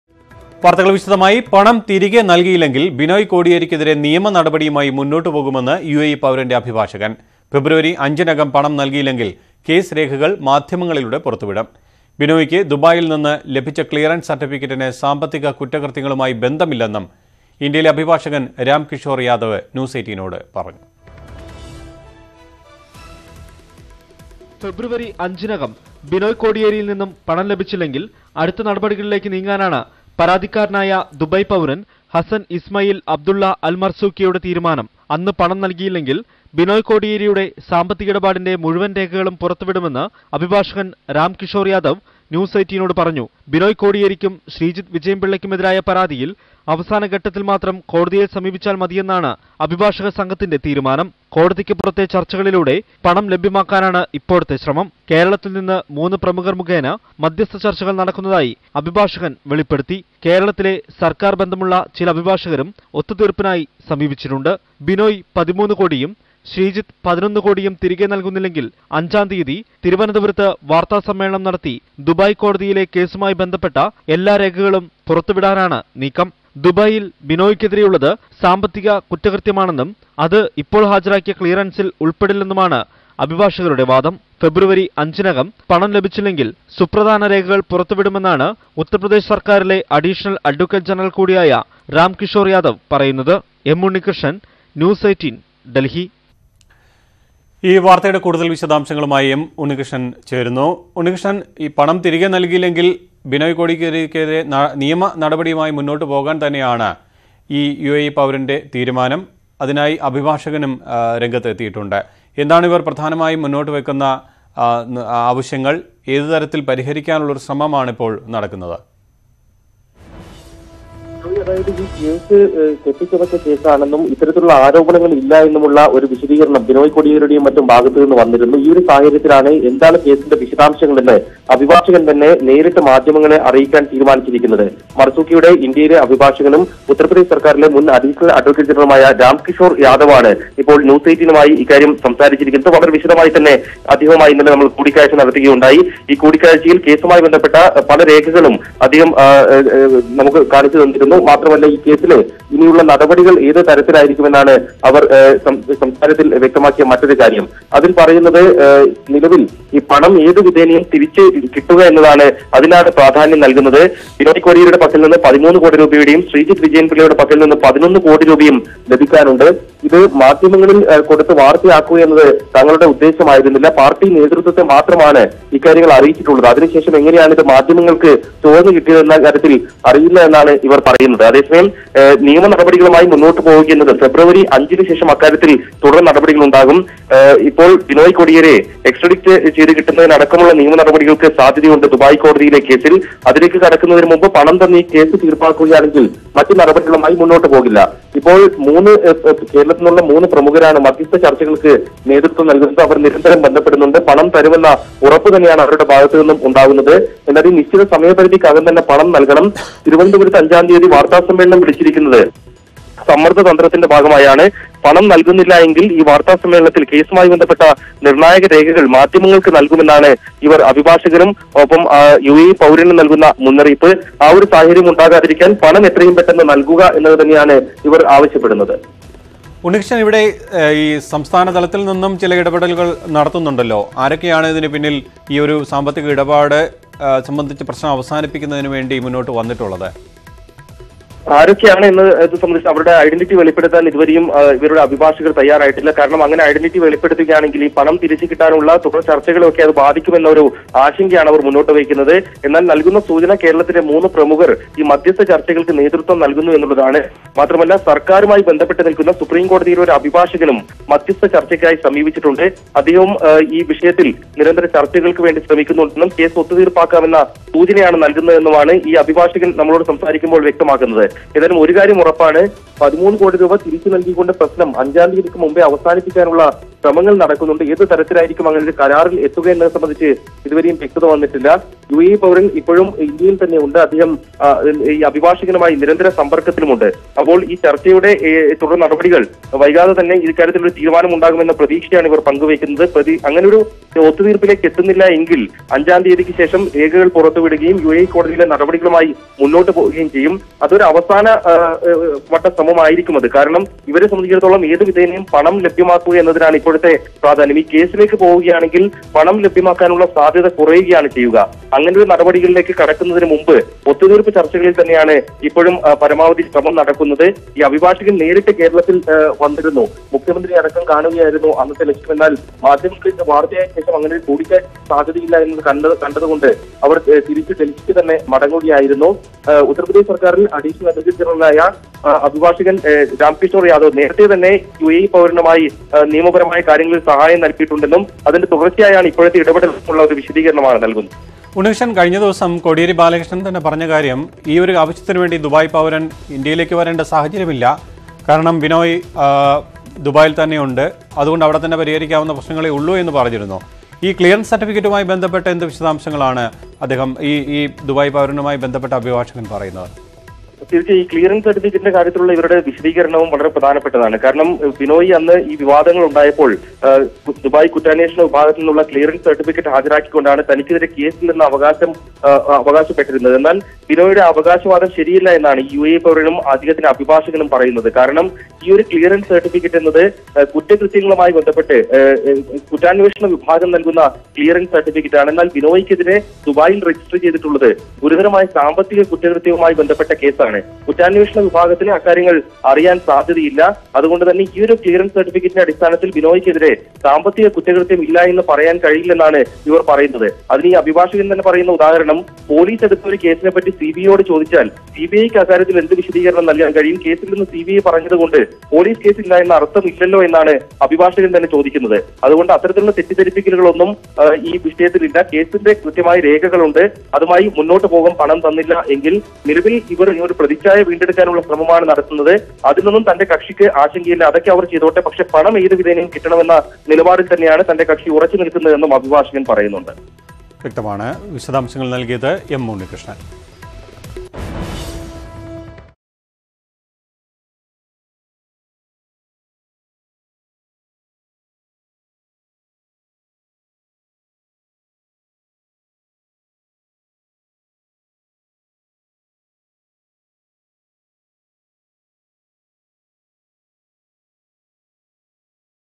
ബിനോയ് കോടിയേരി പണം തിരികെ നൽകിയില്ലെങ്കിൽ നടപടികളുമായി മുന്നോട്ടുപോകുമെന്ന് UAE പൗരന്റെ അഭിഭാഷകൻ
Dubai firm has set 5 February as the deadline to Binoy Kodiyeri for settle dues.